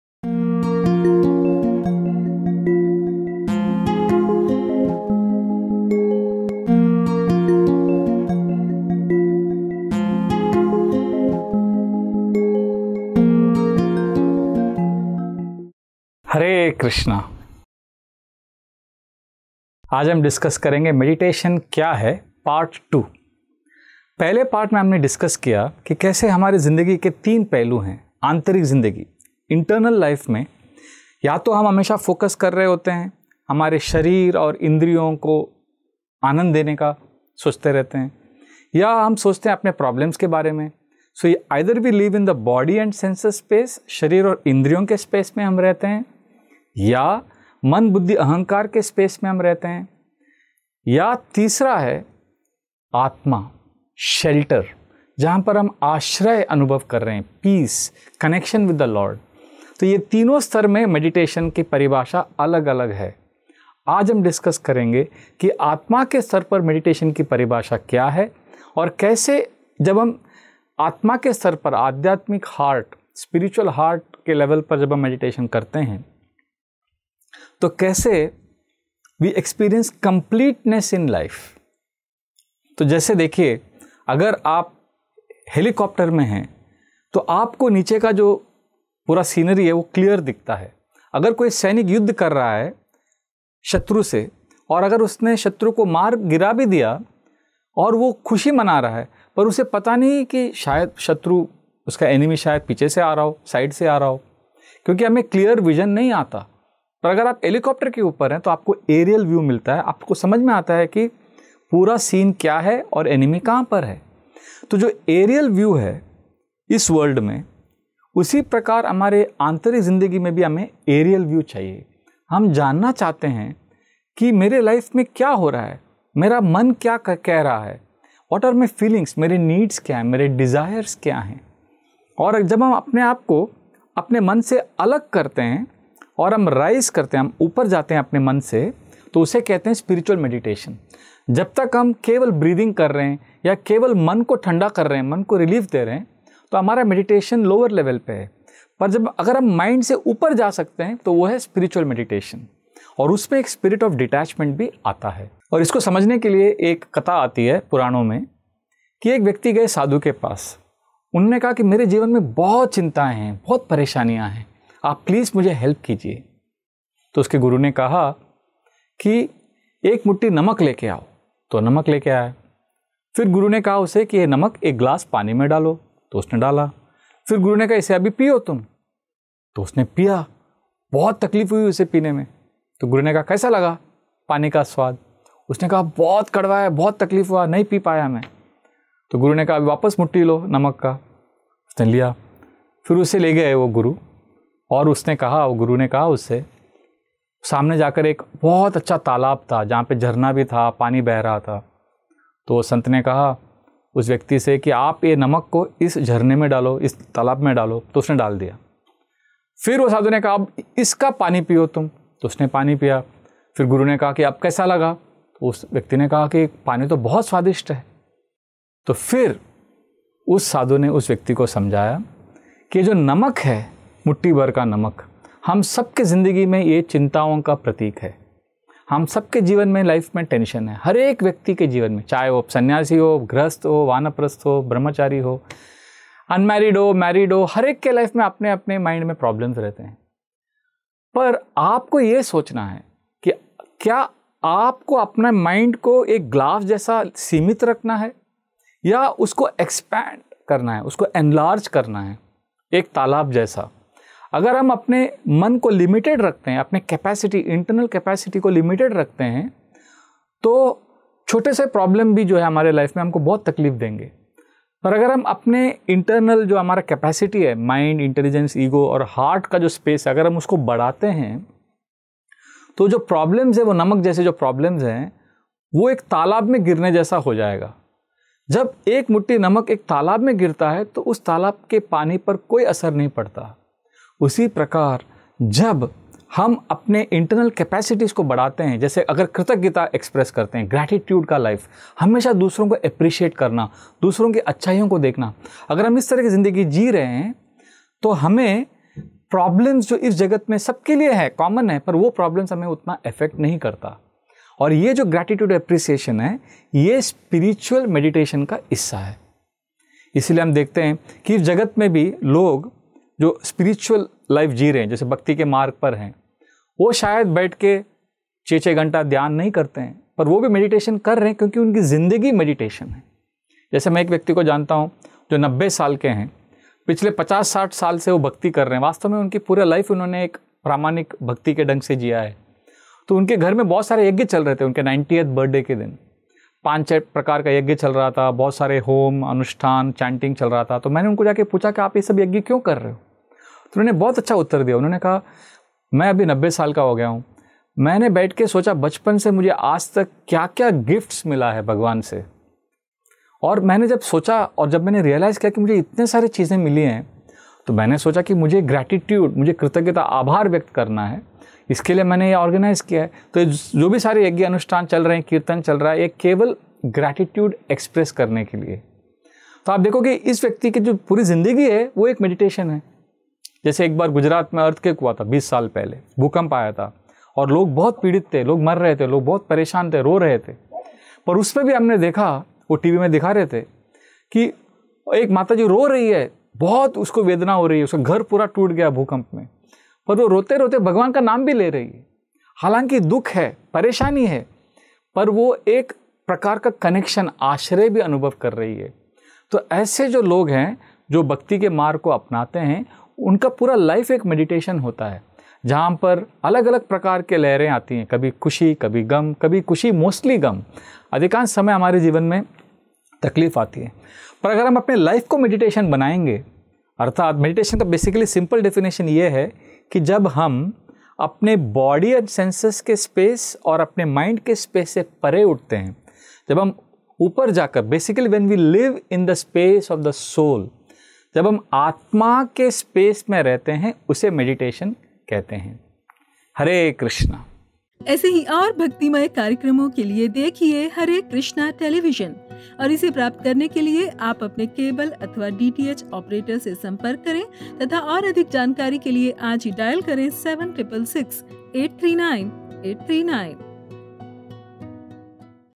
Meditation क्या है, कितने तरीकों से किया जाता है? जानिए कौन-सा है बेस्ट | Vraj Bihari Prabhu | Ep 2
Meditation क्या है, कितने तरीकों से किया जाता है? जानिए कौन-सा है बेस्ट by Vraj Bihari Prabhu in Episode 2
of the series. Meditation is an important topic that many people are curious about. In this video, we are going to discuss about the benefits of meditation, its real meaning and how to meditate.